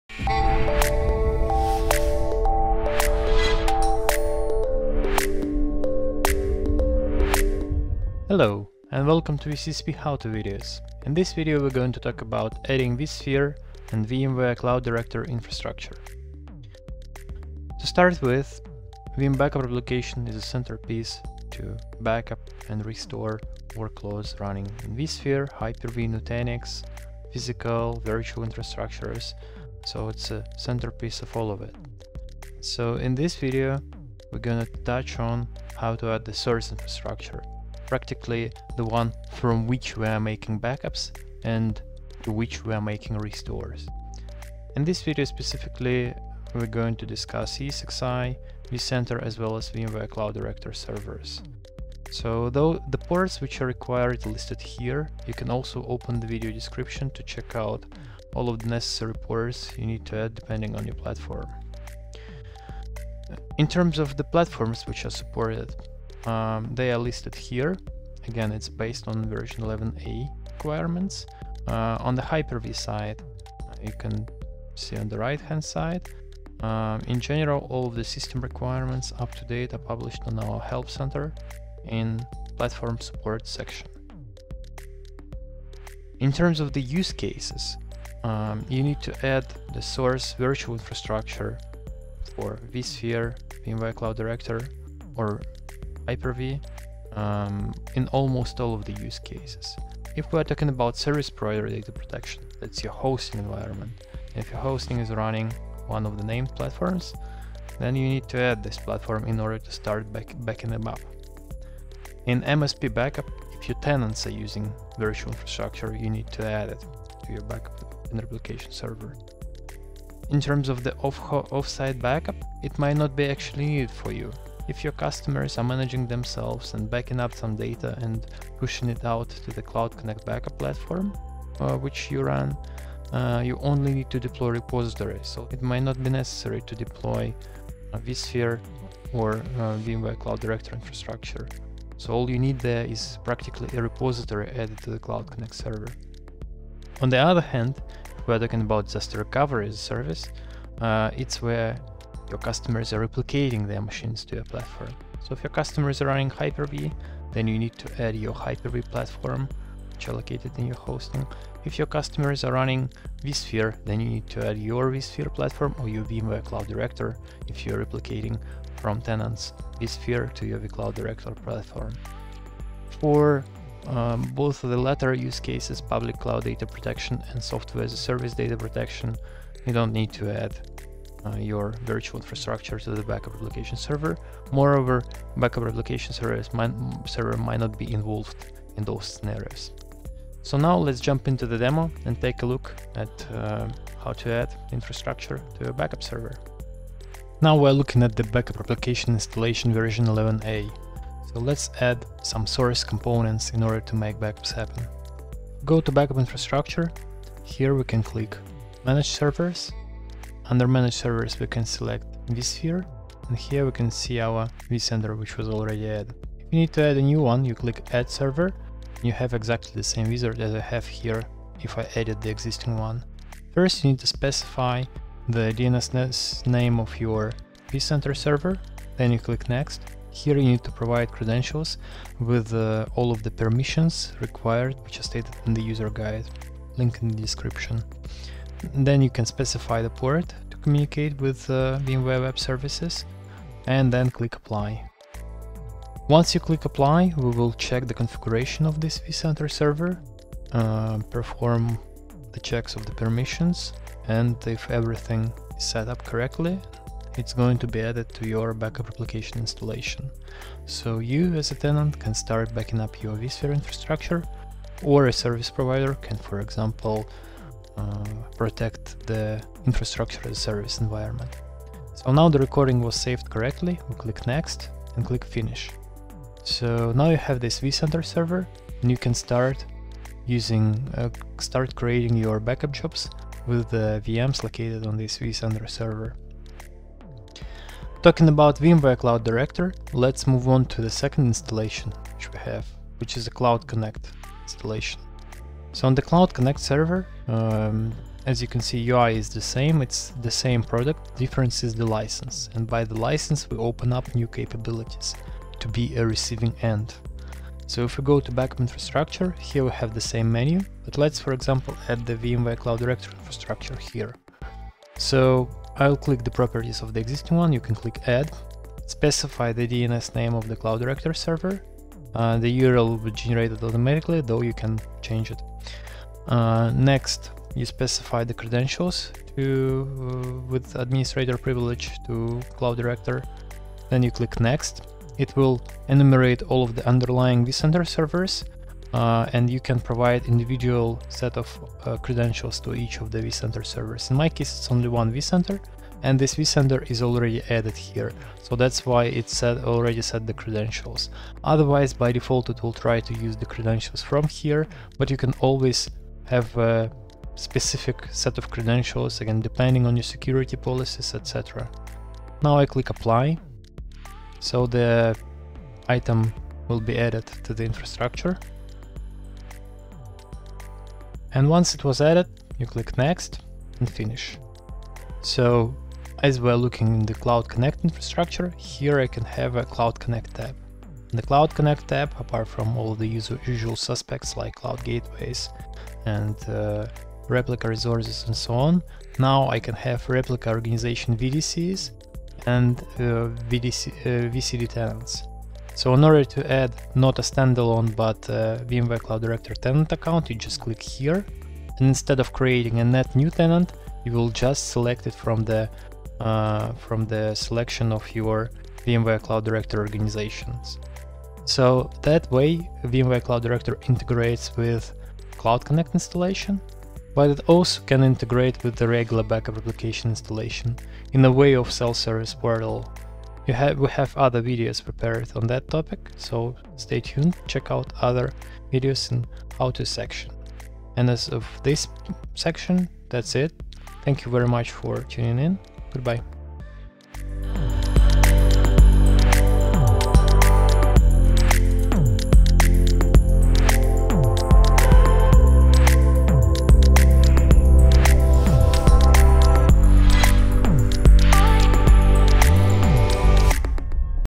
Adding a VMware vSphere/Cloud Director infrastructure
In this how-to video, Veeam® experts demonstrate how to add a VWware vSphere/Cloud Director infrastructure.

More information for what platforms Veeam Backup & Replication™ supports can be found